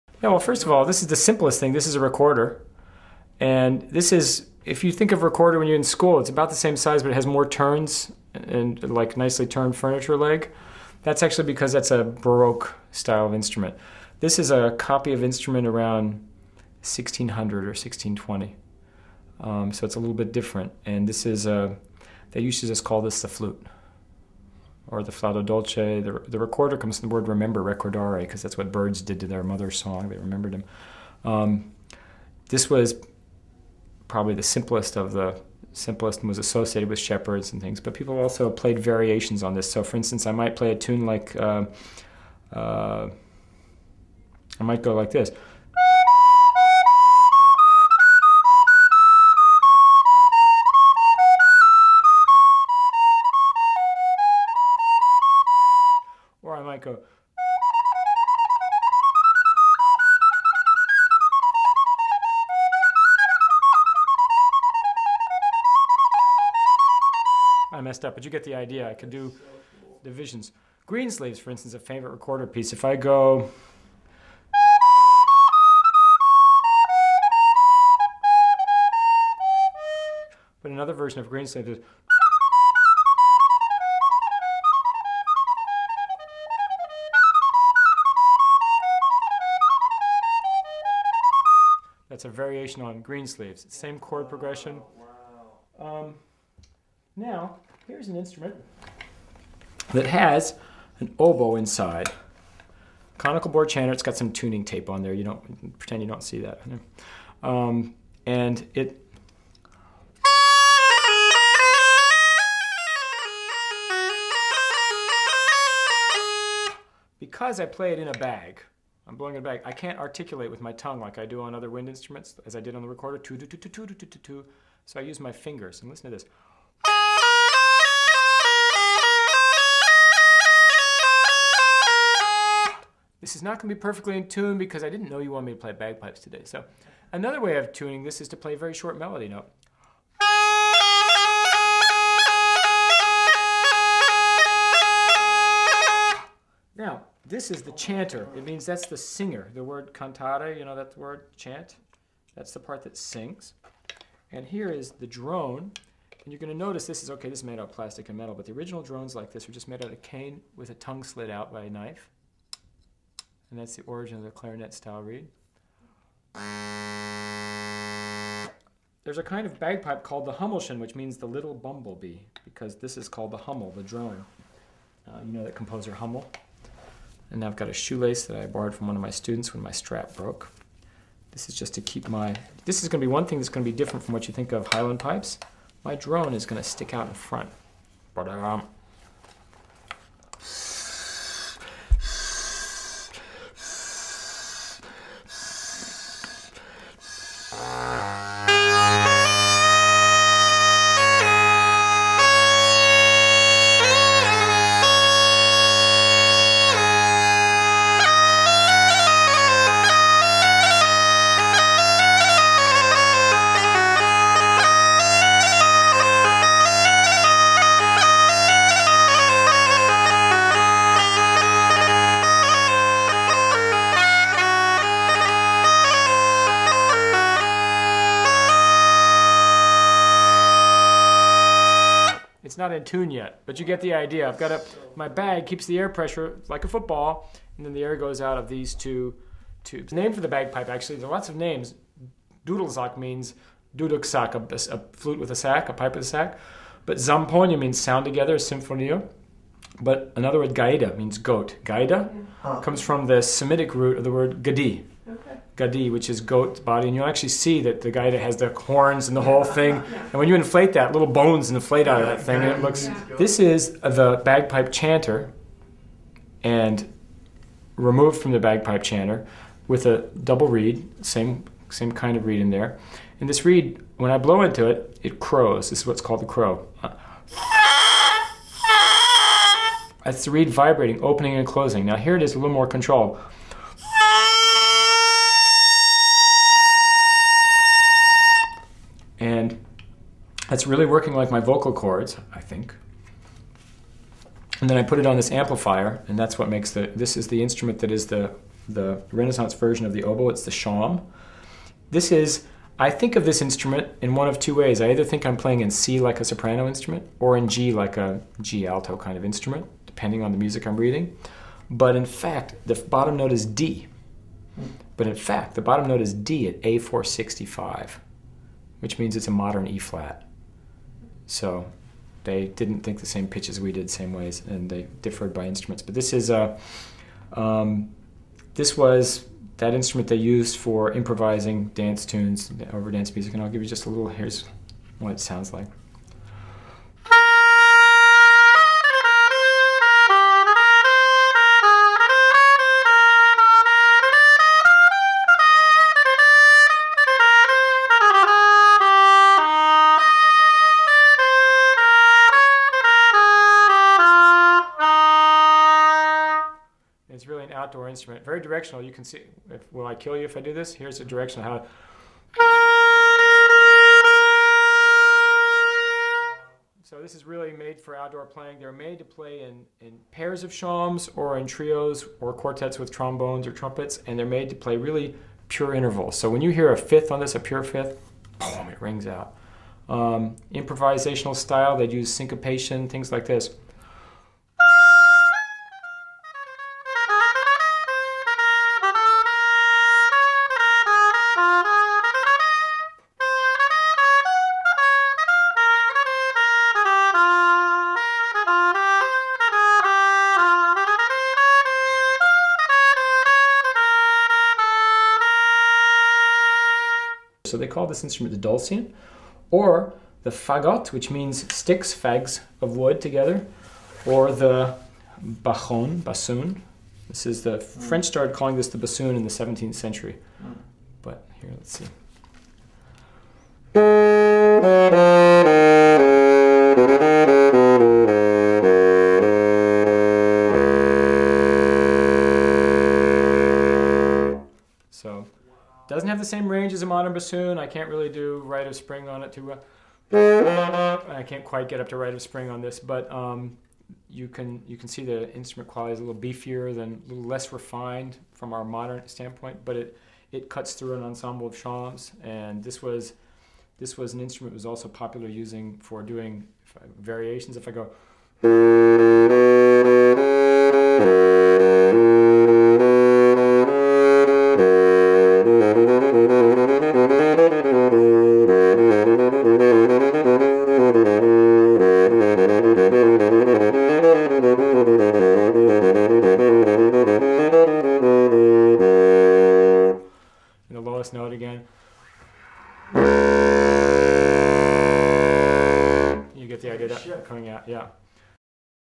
An Introduction to Early Music Instruments, Part 1 of 2: Recorder, Bagpipe and Shawm
- Adam Gilbert of the University of Southern California introduces and demonstrates the playing of some commonly encountered Early Music instruments, including the recorder, bagpipe, and shawm. Part 1 of 2.